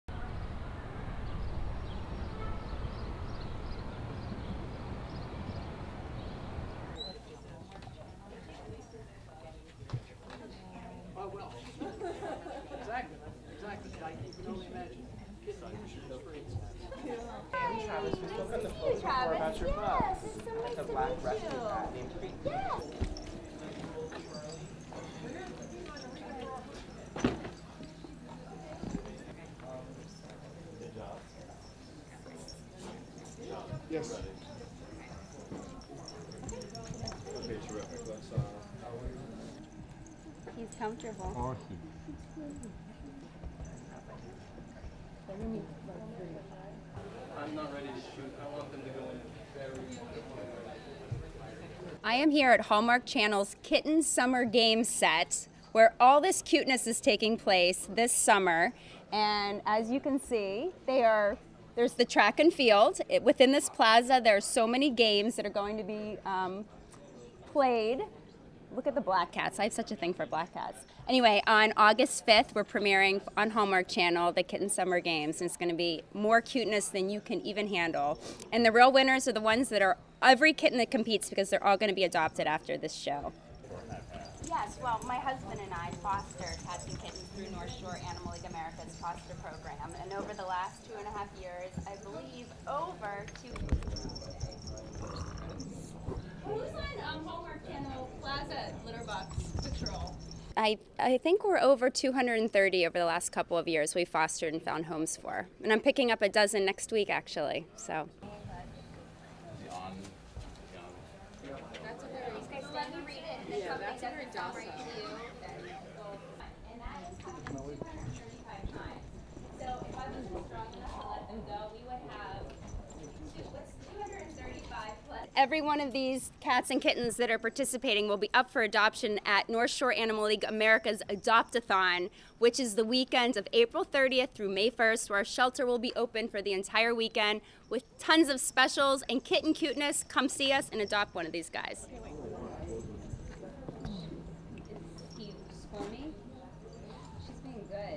Kittens go for gold in 'Summer Games' in Hallmark Channel special
(20 Apr 2016) ADOPTABLE AND ADORABLE KITTENS RUN, JUMP, AND RACE IN SUMMER GAMES
What do the four-legged footballers of the Hallmark Channel's "Kitten Bowl" do in the offseason? They go for the gold in their own "Summer Games," including a de-cat-hlon and freestyle wrestling.
And, bonus, the 90 or so furry Olympians were up for adoption by the rescue group North Shore Animal League America, organizers said Tuesday at the taping of the TV special to air Aug. 5, in time for the human games in Rio.
The cuteness quotient was off the leash as the tiny competitors found their footing on bite-size playing fields, frolicking on scratchable rings at the gymnastics "venue" and jumping little hurdles decked out with delectable feathers intended to keep competitors on the move over at track and field.
Escapees were corralled by spotters, who ringed the sets as huge cameras shot the action.
Beth Stern, North Shore's ambassador and wife of shock jock Howard Stern, was on hand. She's a self-proclaimed crazy cat lady, filling her Instagram with the couple's many fosters and six permanent pet cats. She keeps an eye out for special needs cats, sheltering two that are blind, with one that has only three legs due to arrive soon.
"I think we're over 230 over the last couple of years. We've fostered and found homes for. And I'm picking up a dozen next week actually," Stern said.
Stern is a magician in finding homes for her fosters, totalling more than 200 over the last couple of years.
The kitten games, including some full-grown cat "judges" also up for adoption, comes during "kitten season," the time of year that homeless female felines give birth at rapid rates.
In addition to promoting adoption through the Kitten Games, North Shore will hold an adoption marathon starting April 30 and running for 36 hours, straight through to May 1. More than 700 dogs, cats, puppies and kittens will be available.
"Every one of these cats and kittens participating will be up for adoption at North Shore Animal League America's Adopt-athon, which is the weekend of April 30th through May first. The shelter will be open for the entire weekend with tons of specials and kitten cuteness. Come see us and adopt one of these guys," Stern said.